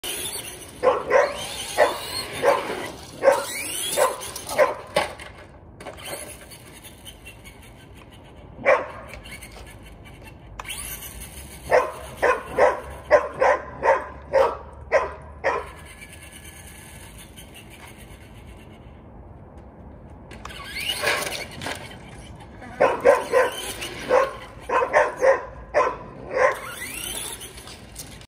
Traxxas Stampede gets Stuck Upright
Watch as this little truck ends up stuck on the gear box and gets helped out by my sister! The Traxxas